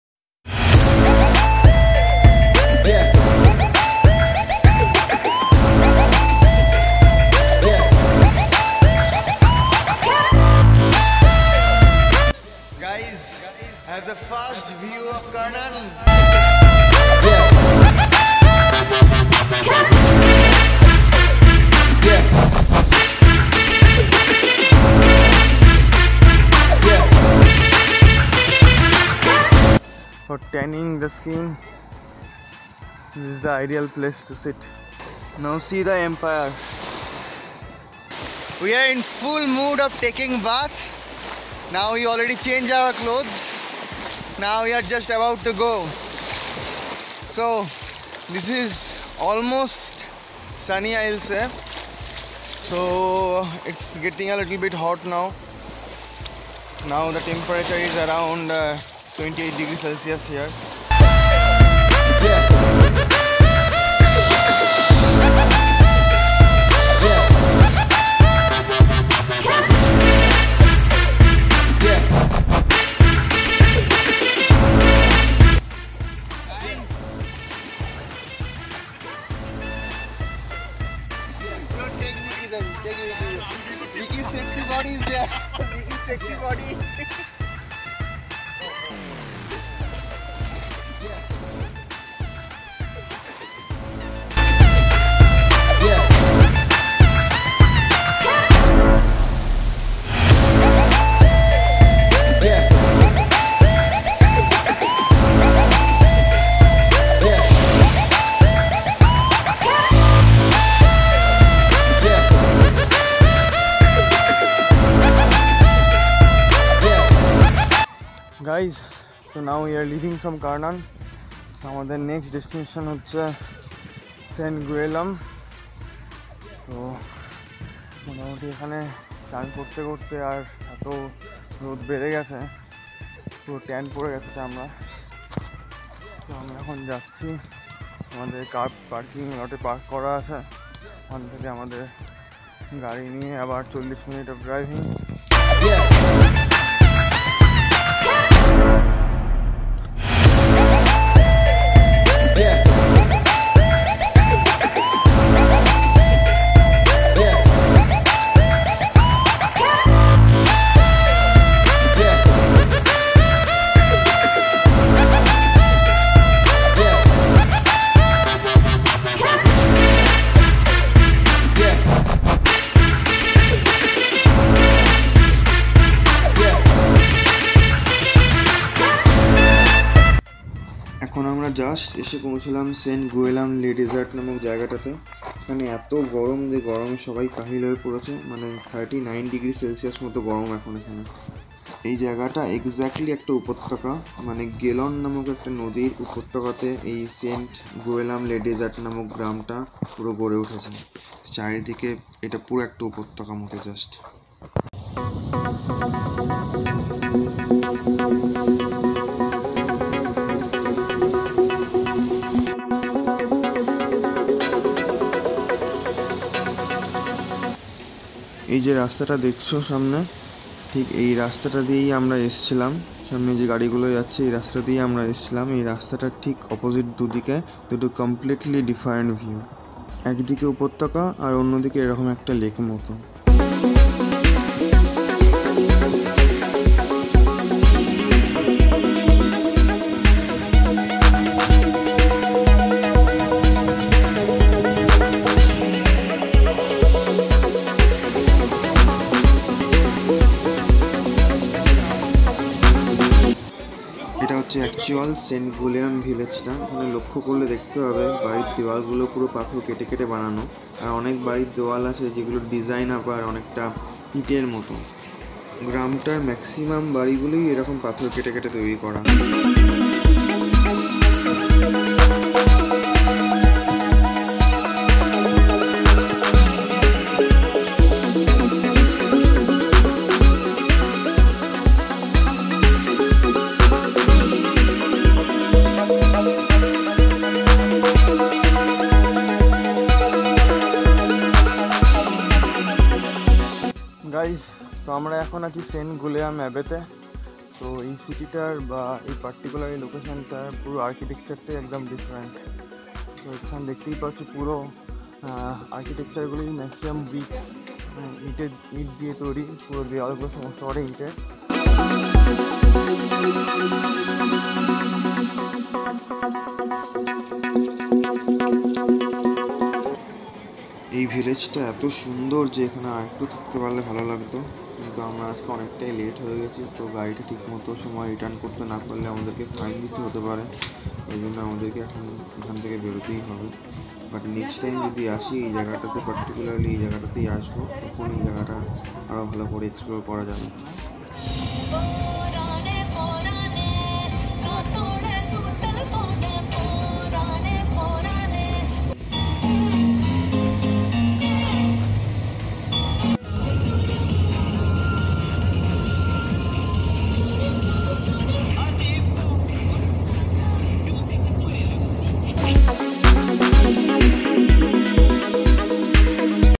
Attractive Sea Beach CARNON - Full Mosti in the Valley in Montpellier - Vlog 28
CARNON is a very beautiful sea beach and Saint Guilhem is the valley of the river called Gellone.